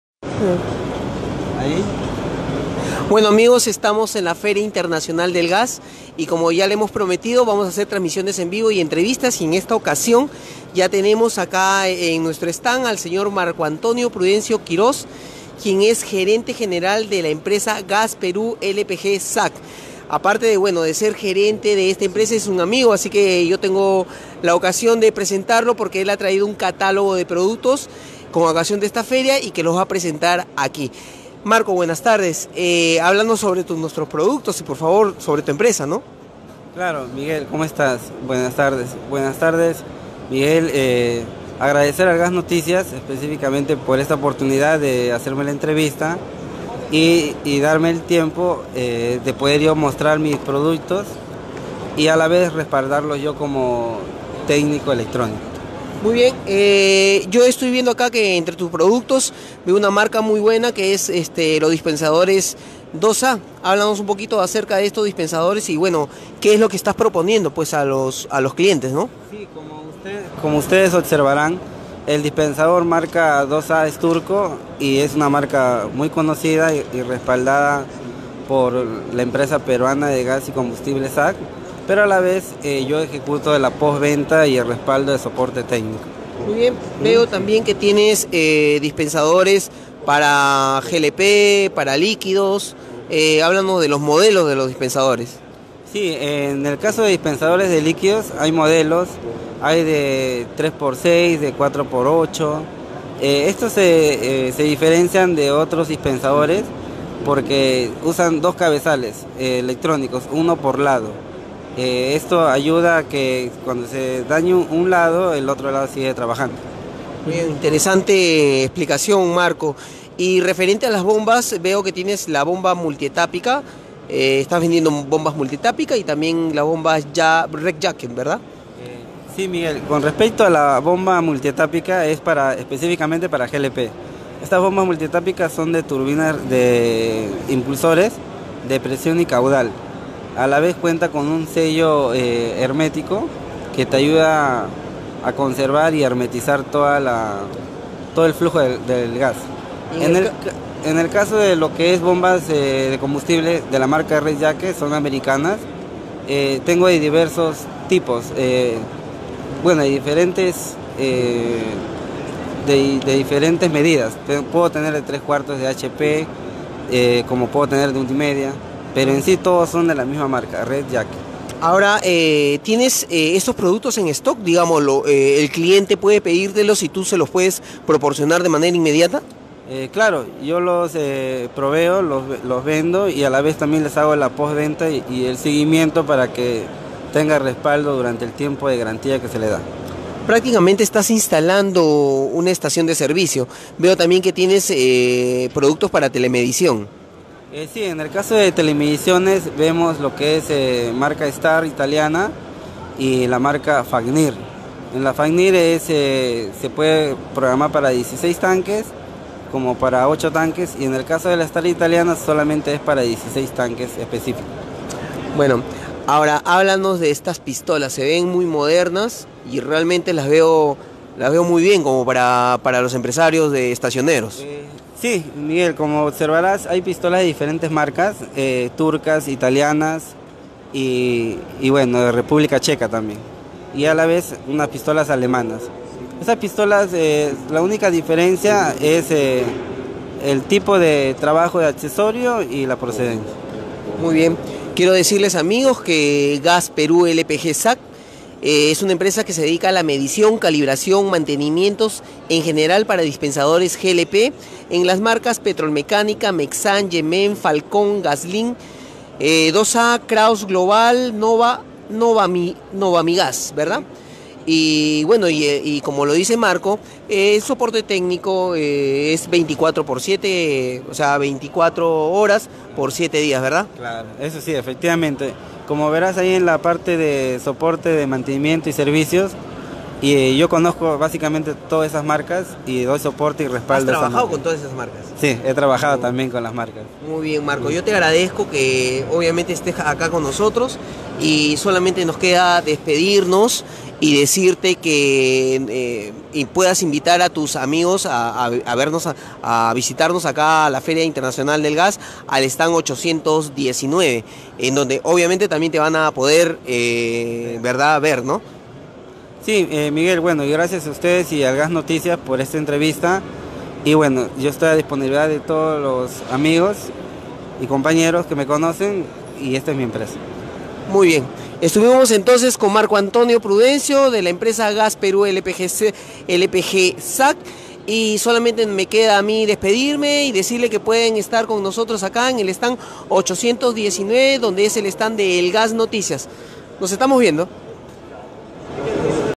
Empresa Gas Perú LPG SAc
Venta de suministros para estaciones de servicio y equipamiento para plantas de envasado. Ingeniería y proyectos de grifos.
Dipsensadores, electromobas y pistolas Falcón